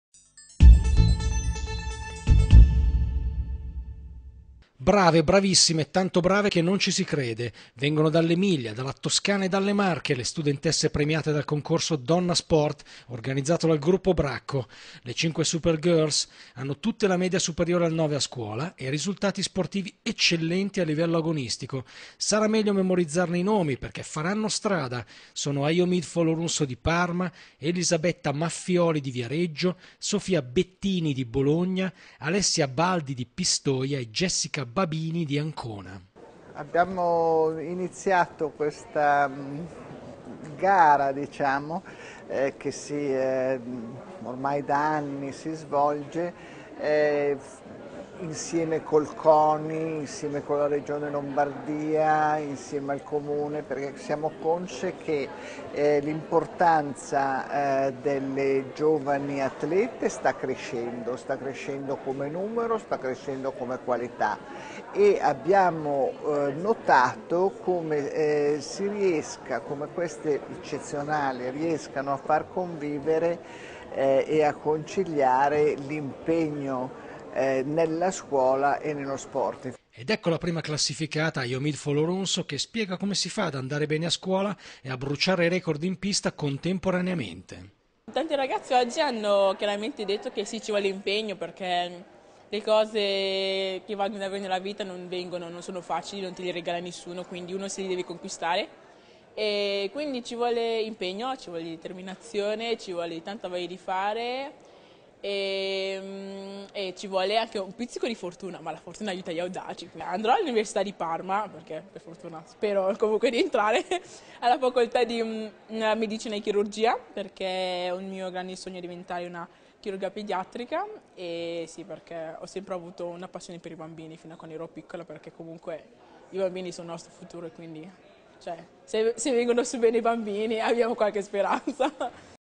Premiazione Donna Sport - L’Atleta più Brava a Scuola
E’ giunta al termine la terza edizione del concorso nazionale: “Donna Sport - L’Atleta più Brava a Scuola”, promosso dal Gruppo Bracco e dedicato alle giovani sportive della scuola media superiore che riescono a portare avanti gli studi con un'ottima media e un brillante curriculum sportivo. L’iniziativa, che rappresenta il cuore del progetto Donna Sport, è stata ideata e realizzata da Bracco per valorizzare la figura della donna in ambito sportivo, diffondendo al contempo tra le giovani i valori positivi dello sport come strumento di crescita personale.